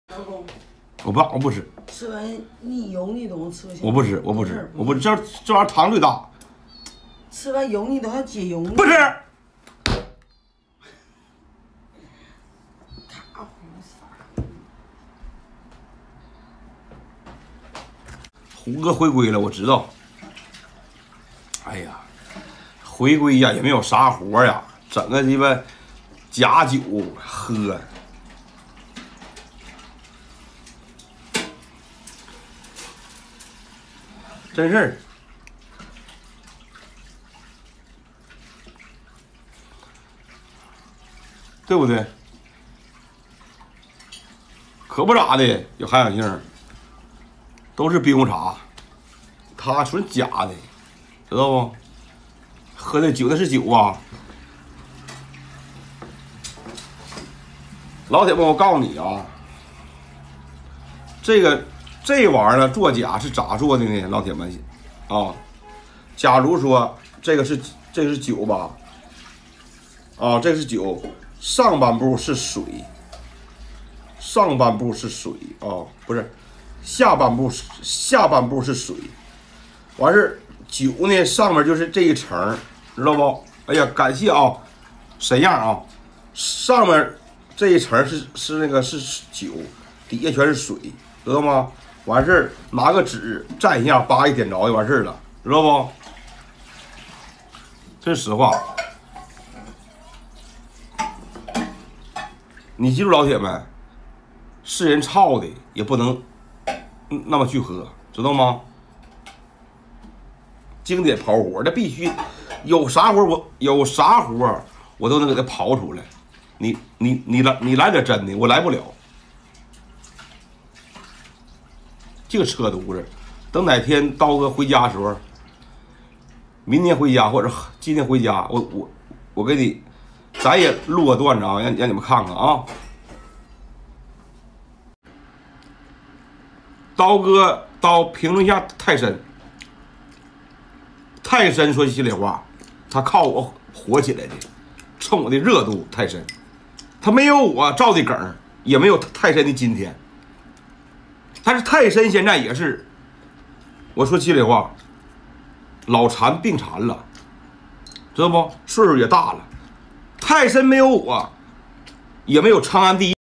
刀哥破案虎哥喝的全是假酒，掺了白开水糊弄粉丝！泰森能火全靠蹭刀母热度！
二次元刀酱